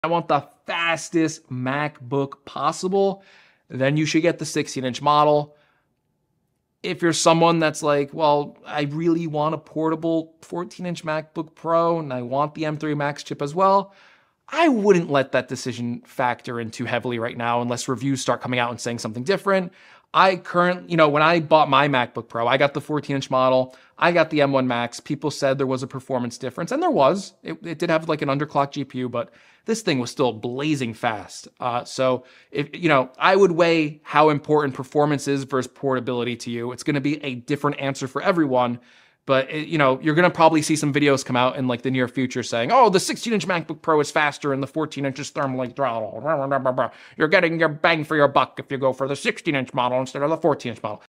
16inch vs 14inch MacBook Pro Choosing Performance over Portability
New M3 Macbook Comparising!